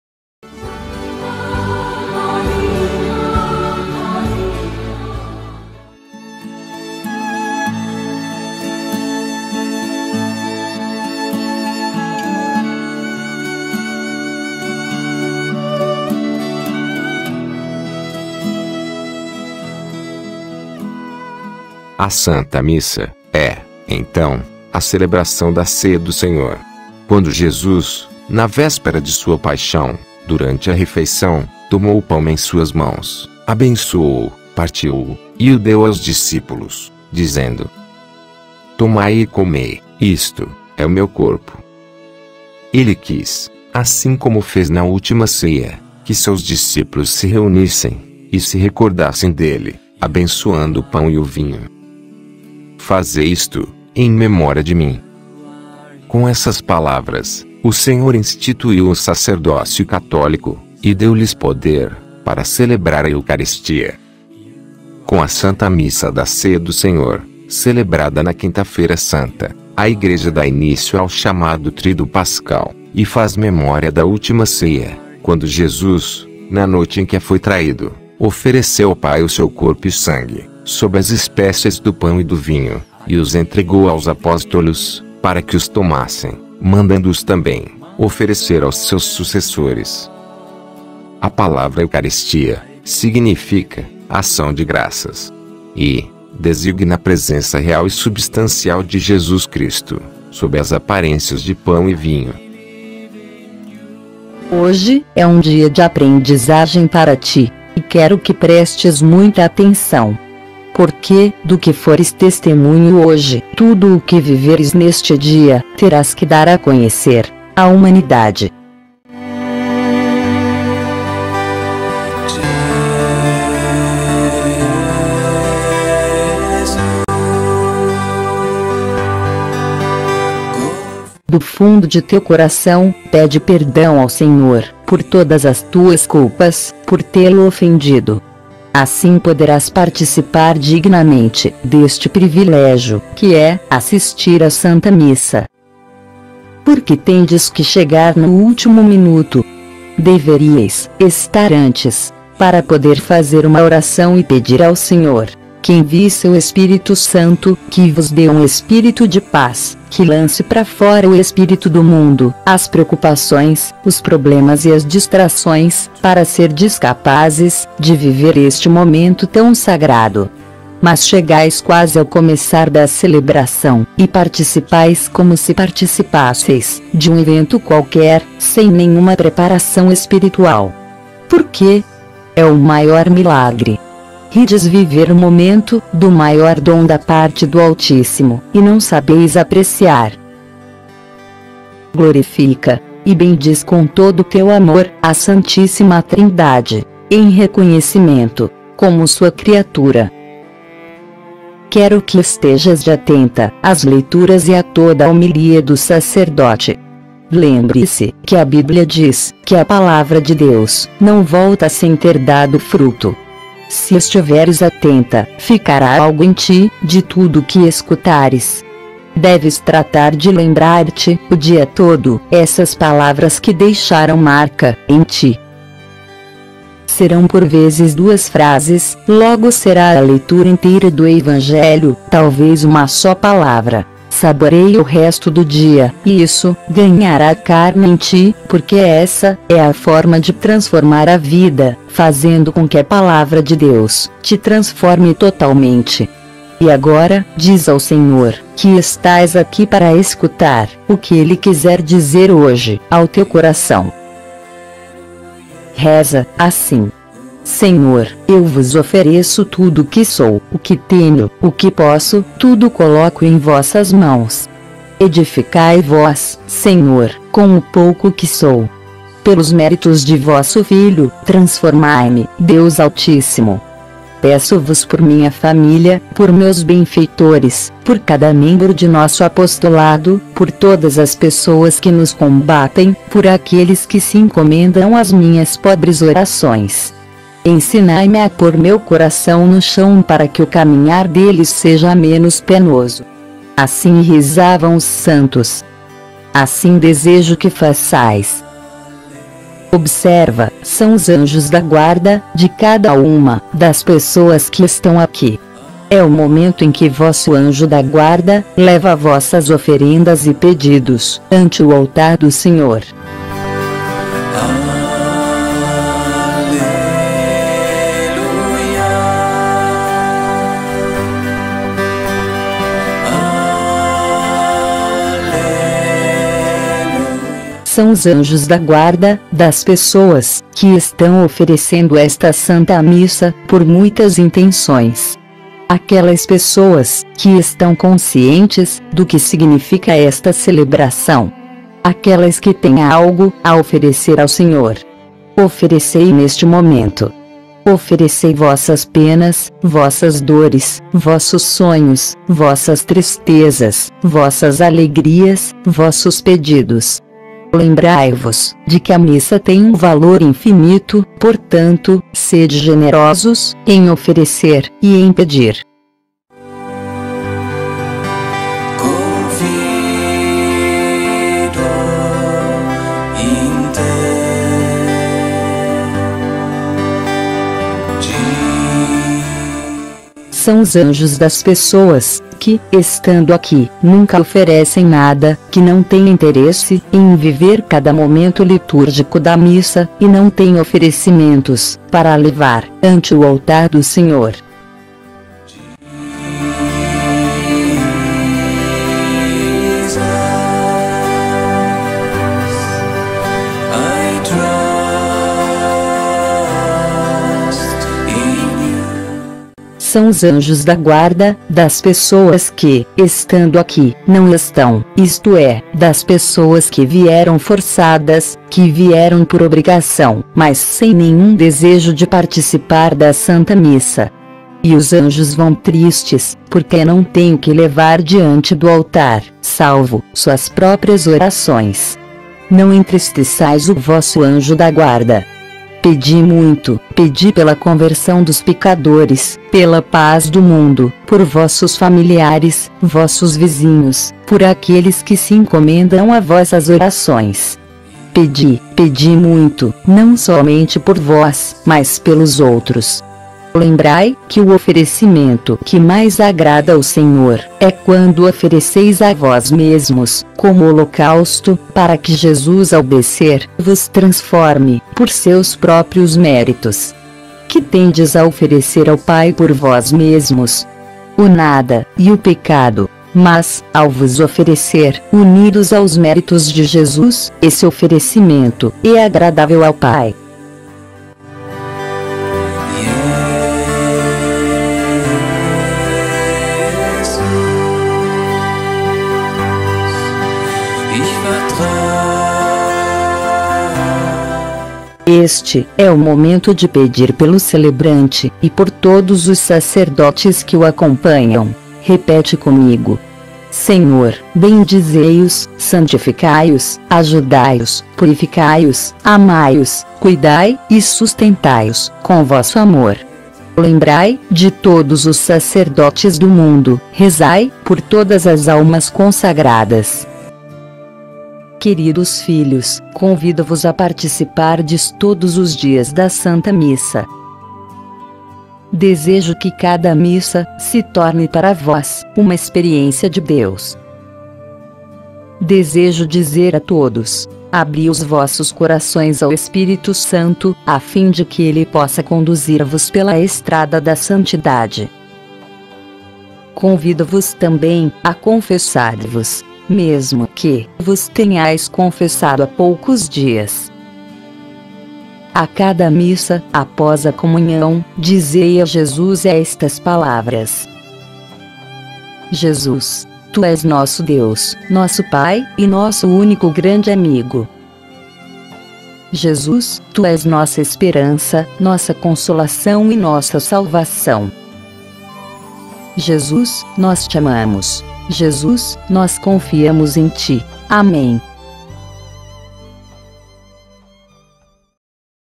Quinta-feira Santa - A SANTA MISSA
Com a Santa Missa da Ceia do Senhor, celebrada na Quinta-feira Santa, a Igreja dá início ao chamado Tríduo Pascal e faz memória da Última Ceia.